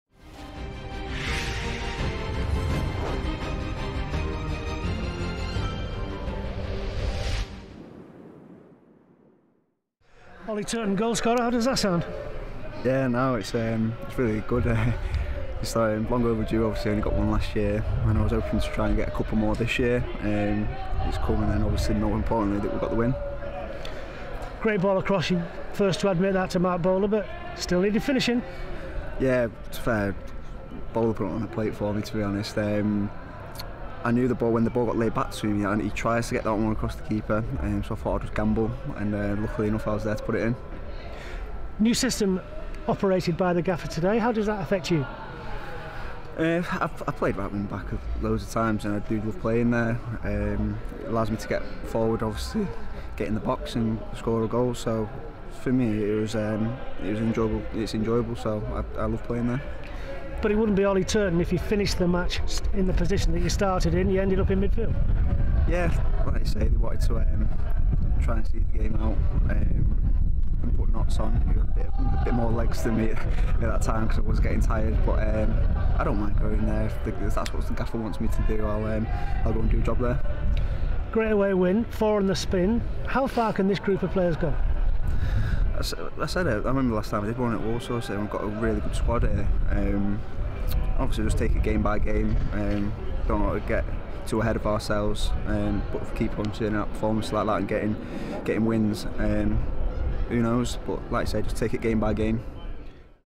Southend Reaction | Ollie Turton
Ollie Turton speaks to the media after scoring in Blackpool's 2-1 victory at Southend United.